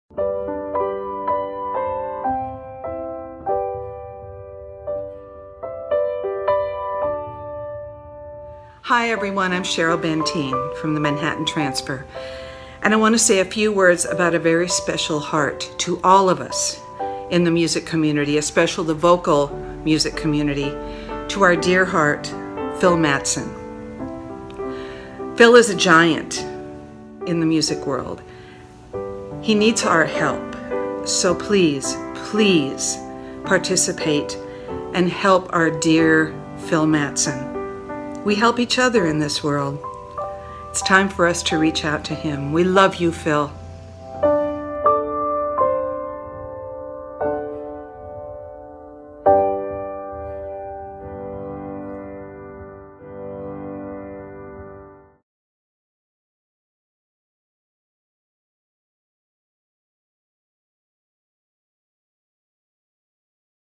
Cheryl Bentyne Support Video for Phil Mattson's GoFundMe campaign.
A video of support from Cheryl Bentyne of the Manhattan Transfer for Phil Mattson. Please visit our campaign and KEEP SHARING. Thanks.